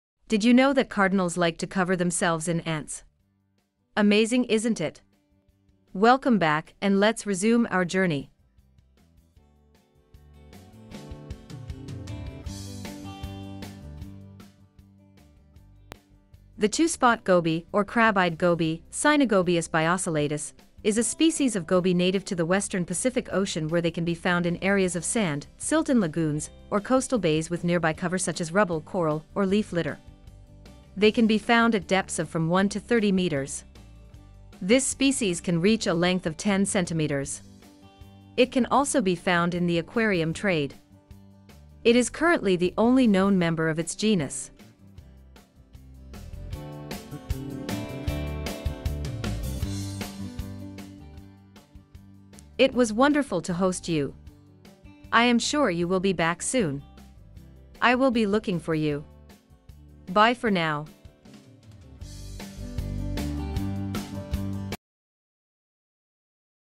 Two Spot Goby facts 🐠 Crab-eyed Goby 🐠 native to the Western Pacific Ocean 🌊
the Two Spot Goby, or Crab-eyed Goby, (Signigobius biocellatus) is a species of goby native to the Western Pacific Ocean where they can be found in areas of sand, silt in lagoons, or coastal bays with nearby cover such as rubble, coral, or leaf litter. They can be found at depths of from 1 to 30 metres. This species can reach a length of 10 centimetres. It can also be found in the aquarium trade. It is currently the only known member of its genus.